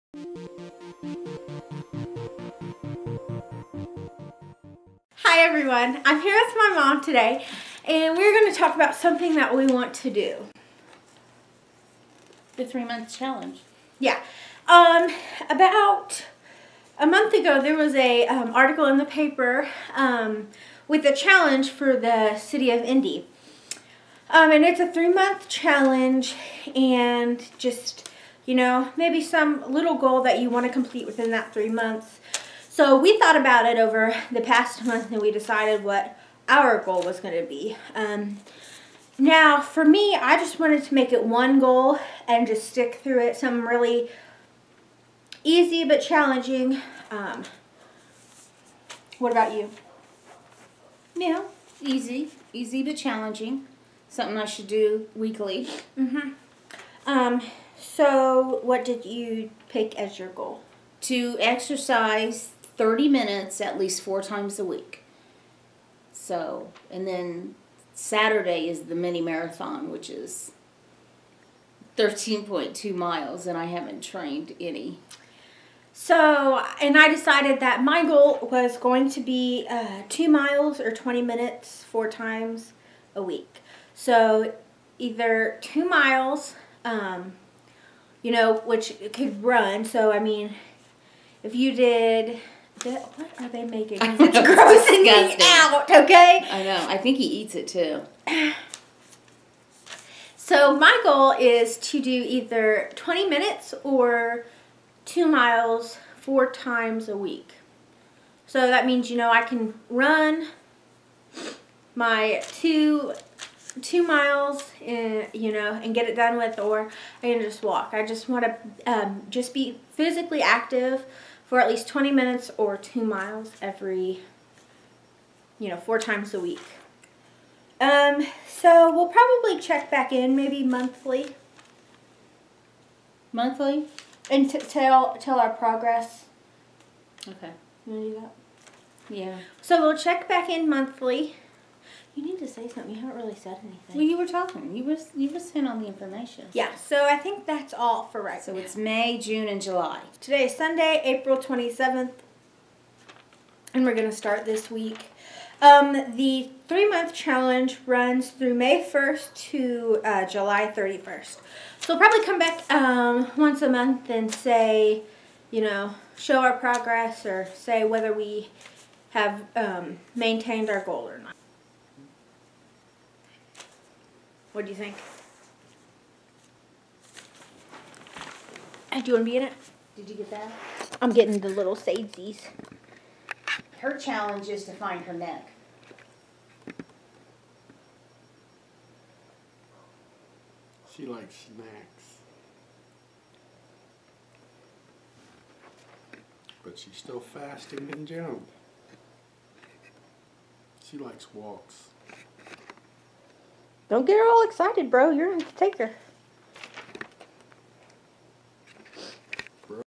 3 Month Challenege Intro
Hi, and thanks for stopping by! In this introductory video my mom and I share our goals that we have set for the 3 month challenge. Look out for more with our monthly check-ins where we'll share our triumps and trials along the way!

The article that insipred us...

Indy's 3 Month Challenge- Will YOU Take It?